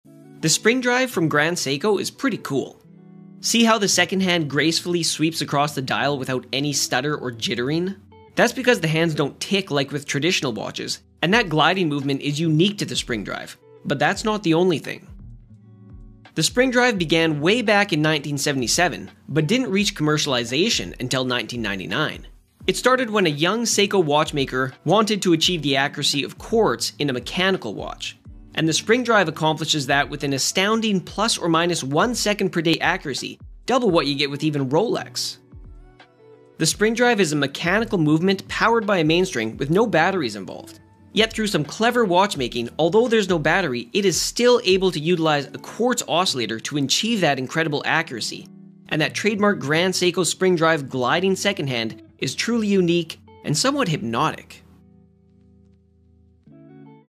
What Is The Grand Seiko Spring Drive? 🔮
➡️ Spring Drive is a unique watch technology. It generates energy like every other luxury mechanical watch but combines this with an electronic regulator to deliver a level of precision that no mechanical watch can match. The development of Spring Drive was possible because Grand Seiko is one of the very few manufacturers with mastery of both electronic and mechanical watchmaking.